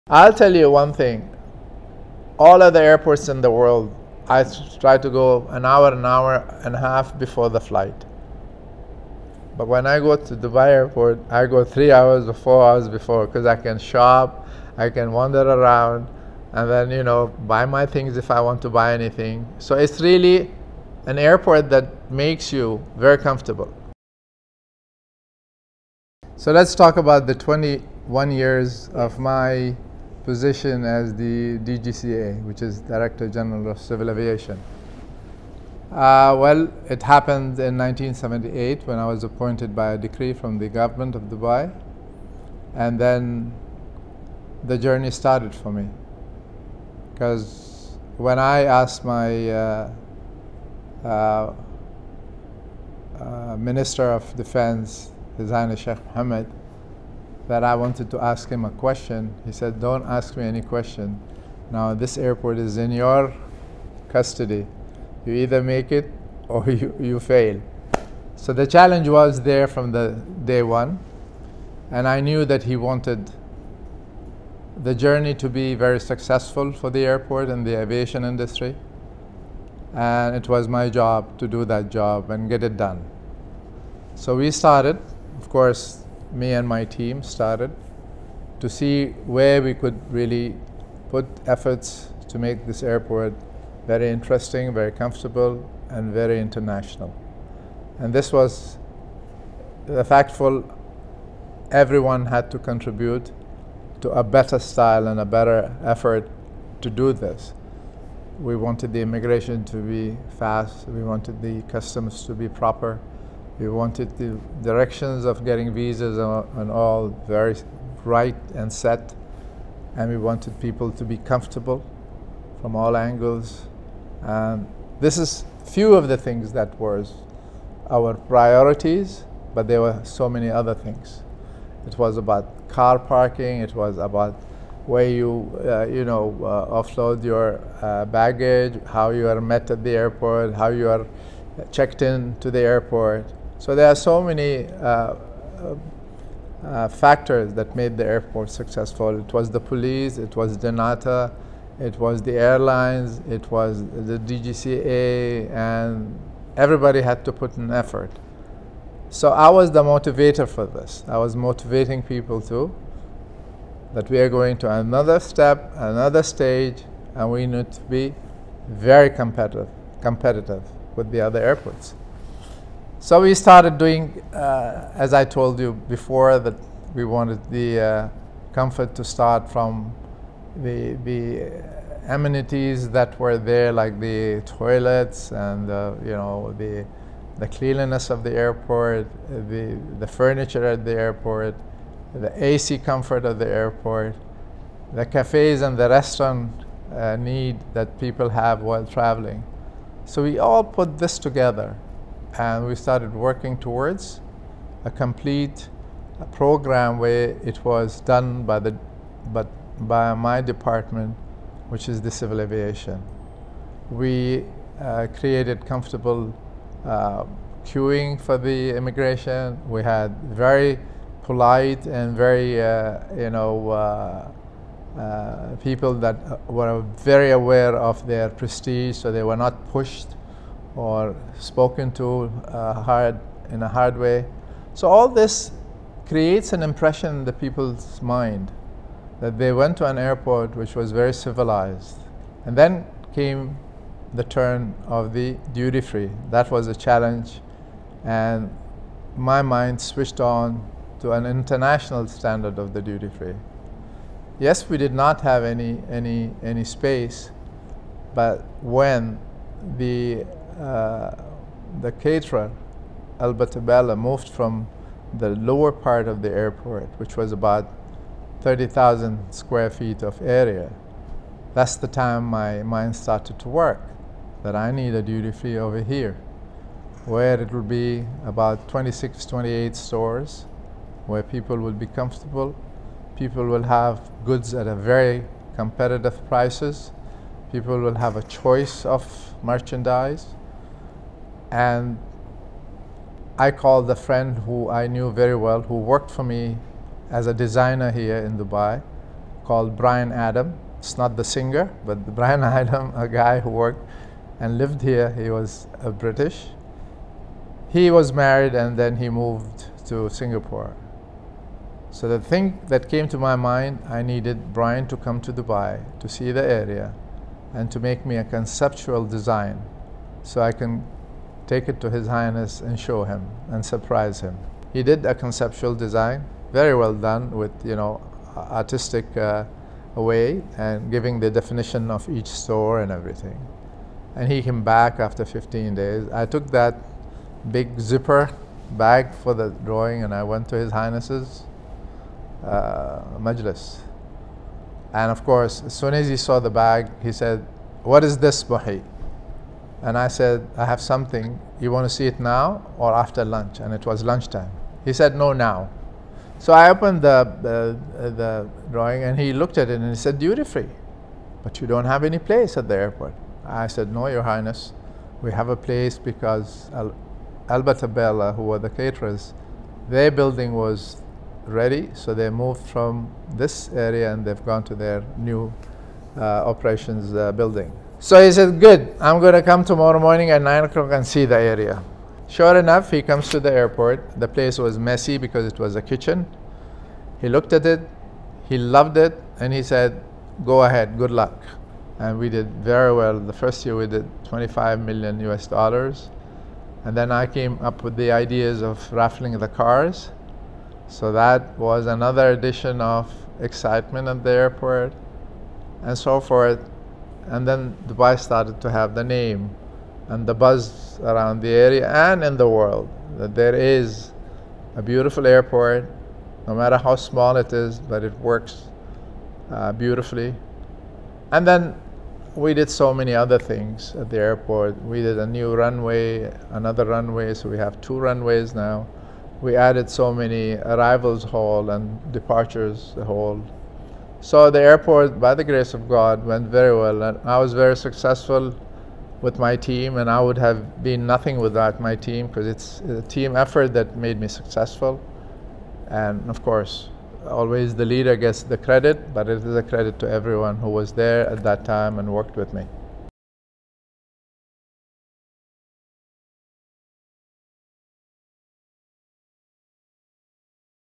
Mohi Binhendi: The man who shaped Dubai Airports
Witty, charismatic, gracious and down to earth yet so larger than life, Mohi-Din BinHendi’s life is the stuff that legends are made of. The former director general of civil aviation (DGCA) - Dubai, now chairman and CEO of BinHendi Enterprises, is the man who helped shape Dubai airport and Dubai Duty Free.